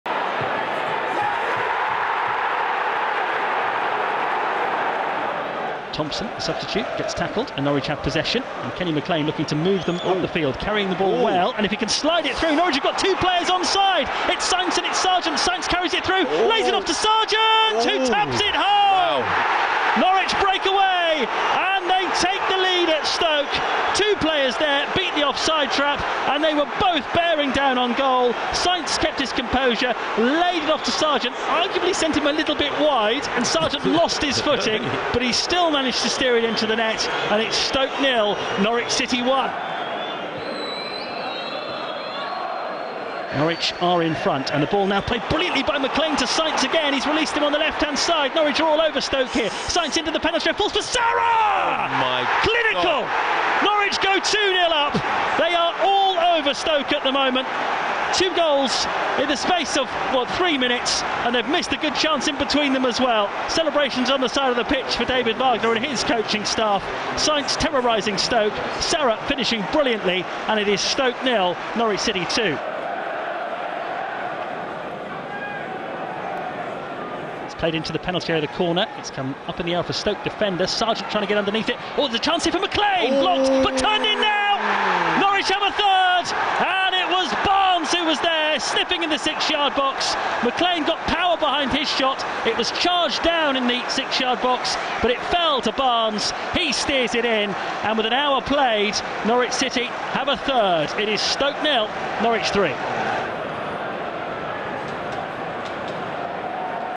ROAD WIN 🚌 | ALL THE GOALS | Stoke City 0-3 Norwich City
Watch all the goals from Sargent, Sara and Barnes as we take all the points at the bet365 Stadium.